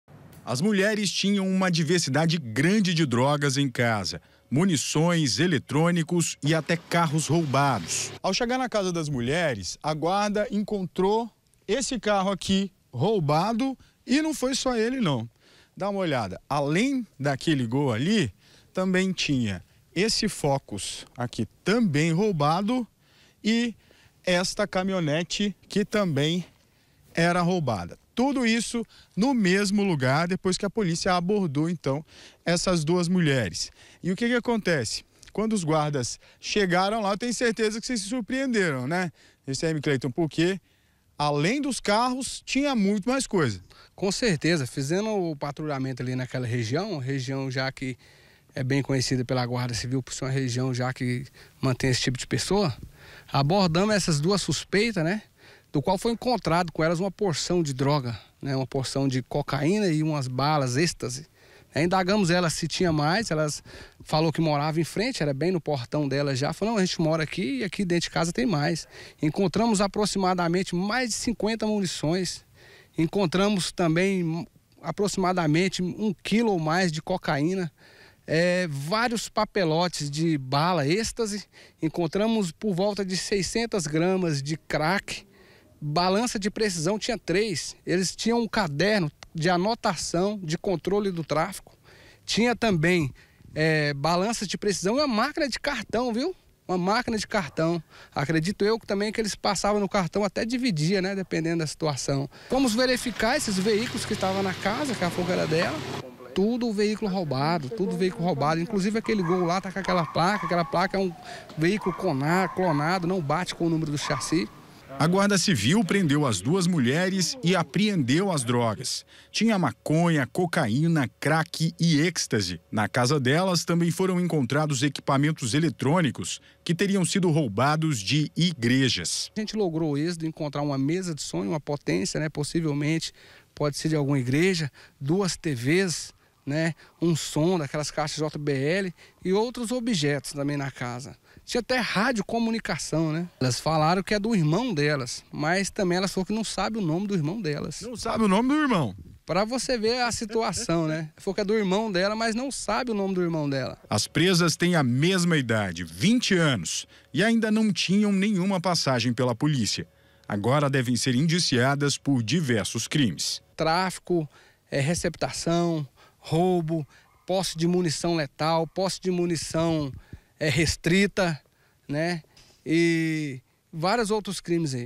OBJETOS ROUBADOS DE IGREJAS SÃO RECUPERADOS
Aparelhos roubados de igrejas, em Senador Canedo, foram encontrados na casa de duas mulheres. Além de aparelhos de som, droga foi apreendida e carros com placas clonadas foram apreendidos com a dupla.